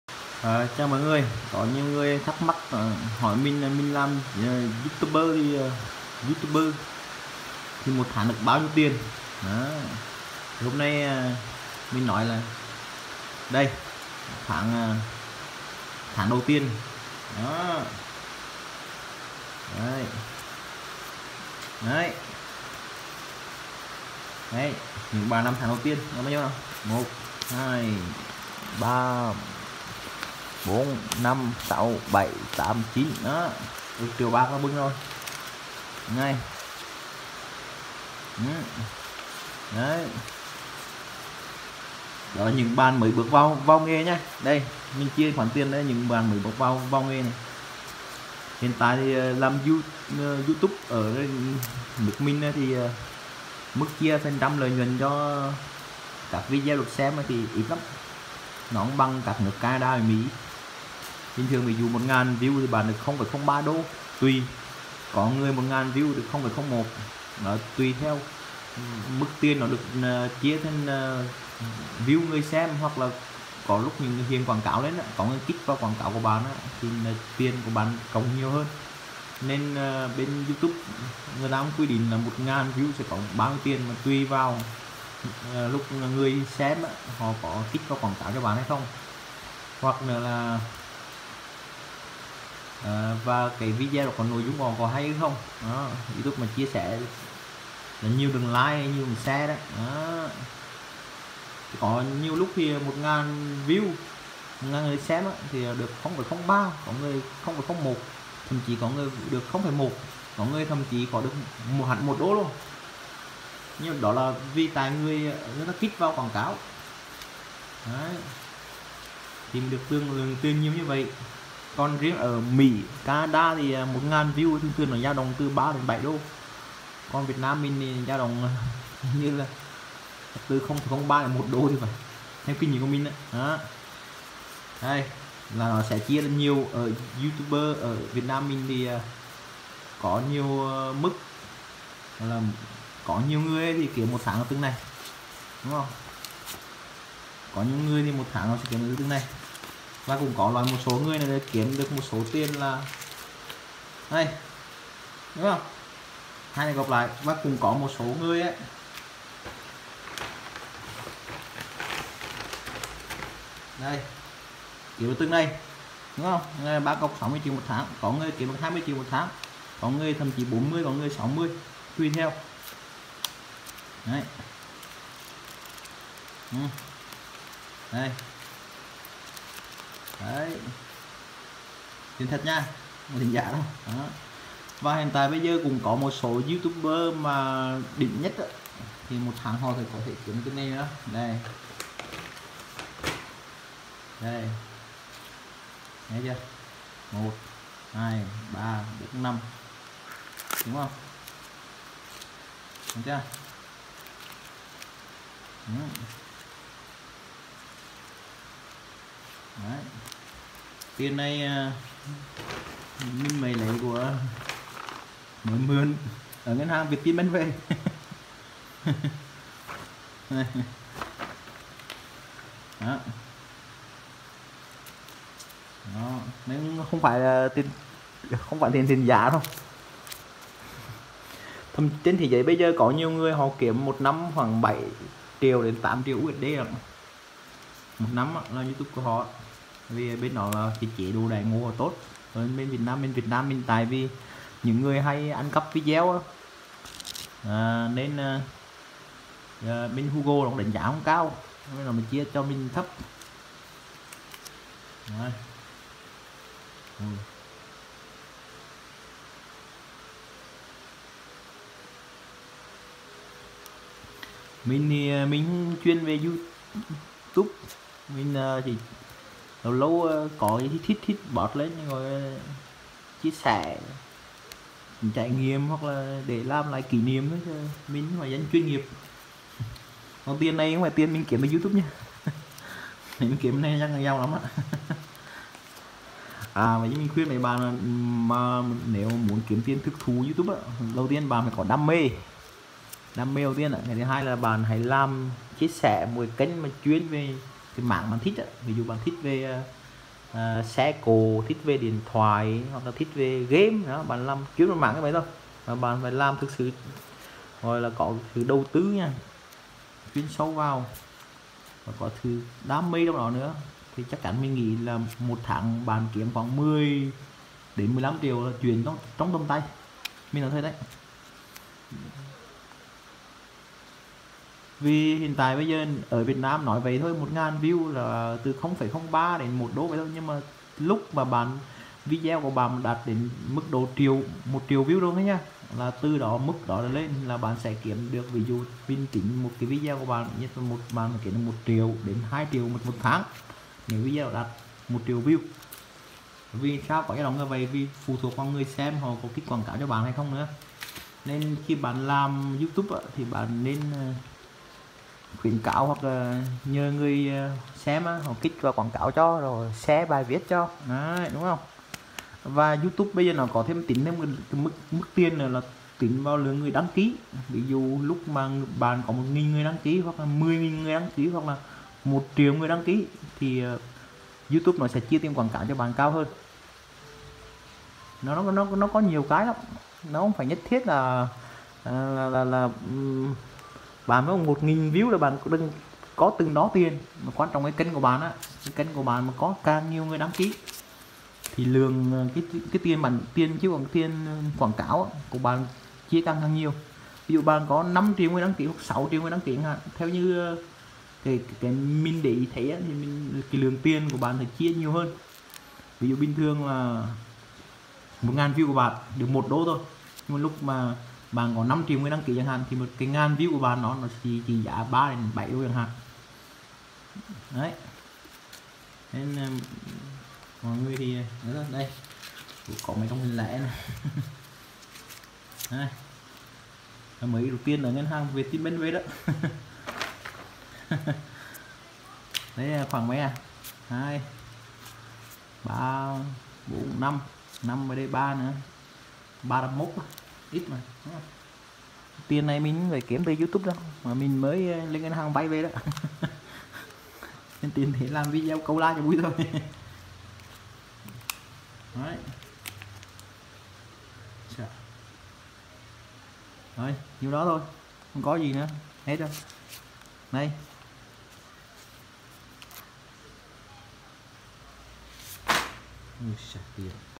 Làm YouTube 1 tháng kiếm được bao nhiêu tiền?
Làm YouTube 1 tháng kiếm được bao nhiêu tiền? Đây là câu hỏi mà nhiều bạn youtuber hay quan tâm nhất. Ngoài ra còn các câu hỏi dạng như:
- 1000 view, like  trên YouTube được bao nhiêu tiền ở Việt NAM.
- cách tính tiền của YouTube ở Việt Nam
- 1 triệu lượt xem kiếm được bao nhiêu tiền trên YouTube.